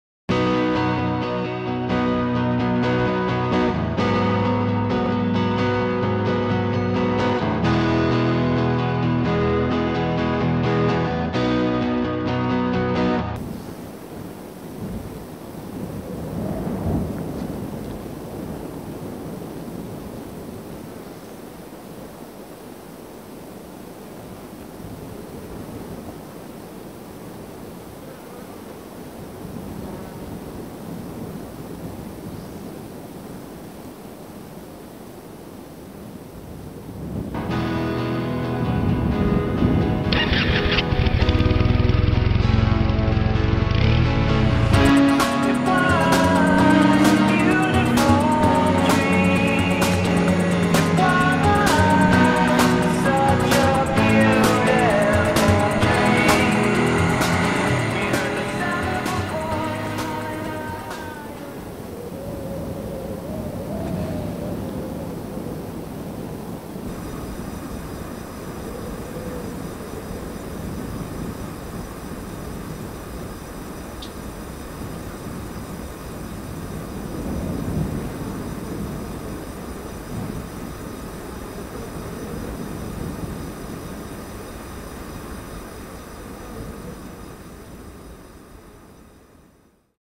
Ride featuring William H Macy
Actor Bill Macy goes for a motorcycle ride in California.